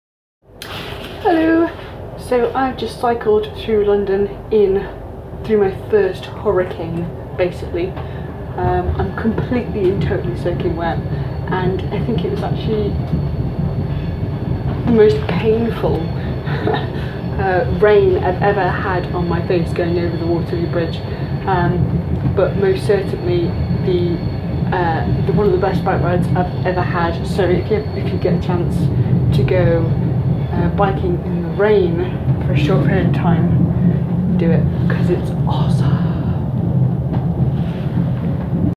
Bike ride through the London hurricane!
Fo serious! Busting with rain!